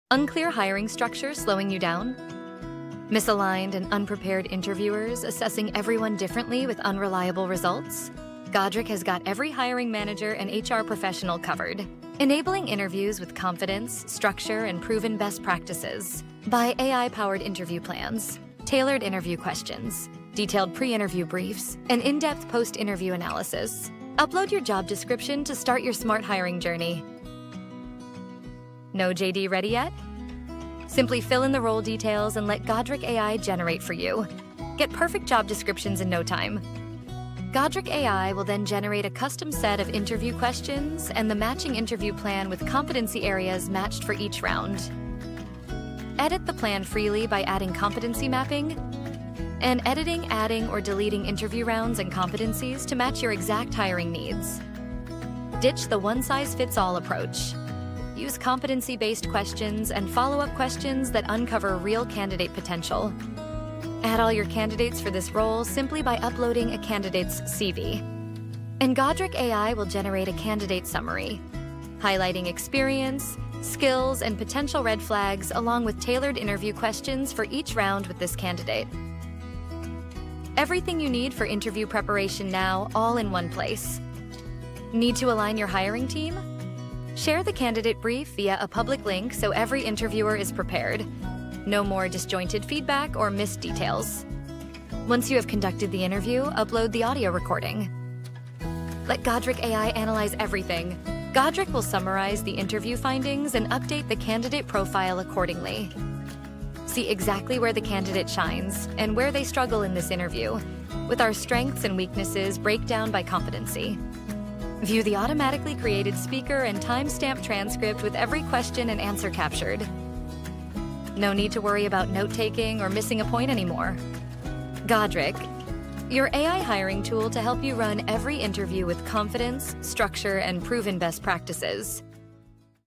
Godric Demo Video – AI Hiring Copilot Walkthrough
See Godric in action! Learn how this AI hiring copilot can streamline your recruiting process, create structured interview plans, and help you hire the right talent with confidence.